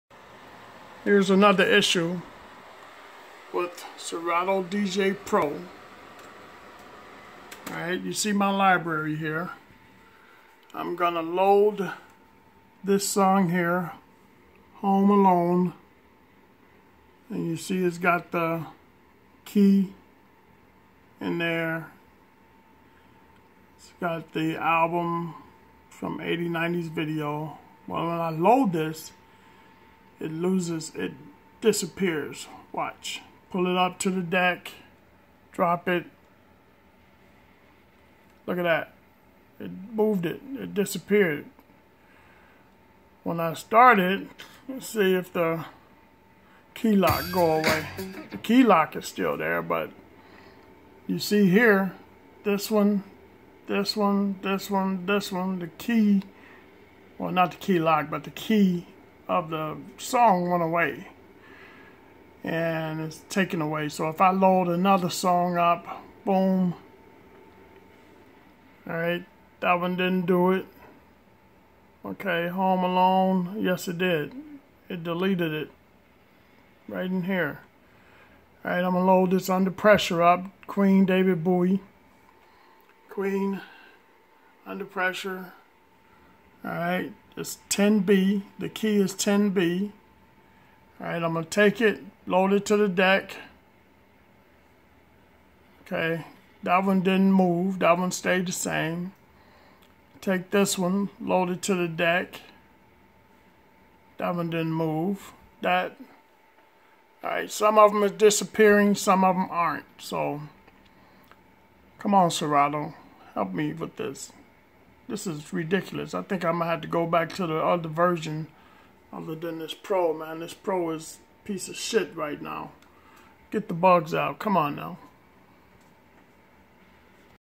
Serato DJ Pro 2.0 obviously has an issue with the videos
2.0 is deleting video information, moving cue points and beat grids. I have lready switch back to the prior version until they fix the bugs